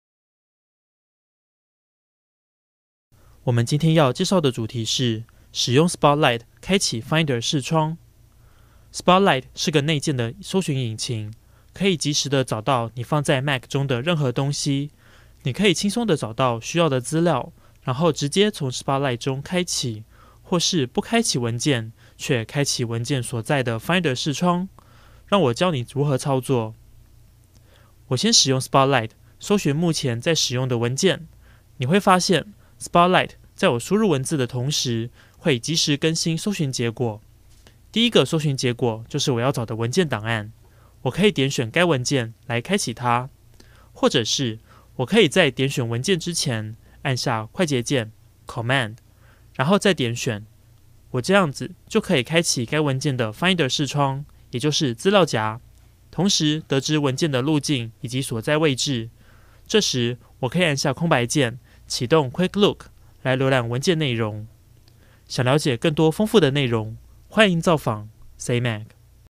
使用 Spotlight 開啓 Finder 視窗
你可以透過Spotlight搜尋並開啓想找的文件資料，要是你先按下快捷鍵Command然後再點選文件，就可以開啓文件所屬的Finder視窗，進而得知該文件的所在位置。